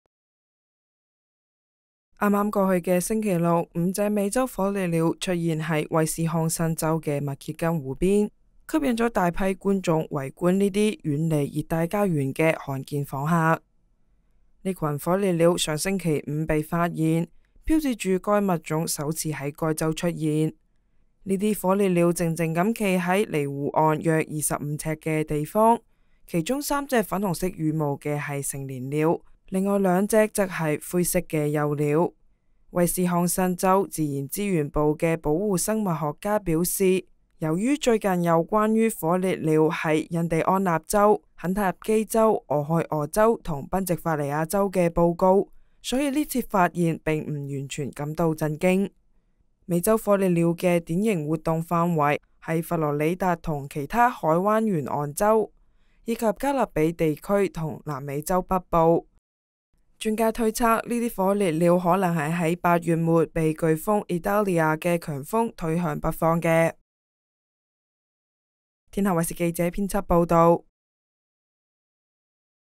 【天下新聞】威斯康辛州: 火烈鳥首次出現 吸引大批觀眾圍觀 American Flamingos Appear in Wisconsin for First Time
剛剛過去的週六，五隻美洲火烈鳥(American flamingo)出現在威斯康辛州的密歇根湖邊，吸引了大批觀眾圍觀這些遠離熱帶家園的罕見訪客。這群火烈鳥上週五被發現，標誌着該物種首次在該州出現。
這些火烈鳥靜靜地站在離湖岸約25呎的地方，其中三隻粉紅色羽毛的是成年鳥，另外兩隻則是灰色的幼鳥。
威斯康辛州自然資源部的保護生物學家表示，由於最近有關於火烈鳥在印第安納州、肯塔基州、俄亥俄州和賓夕法尼亞州的報告，所以這次發現並不完全感到震驚。
美洲火烈鳥的典型活動範圍是佛羅里達和其他海灣沿岸州，以及加勒比地區和南美洲北部。
專家推測，這些火烈鳥可能是在8月末被颶風Idalia的強風推向北方的。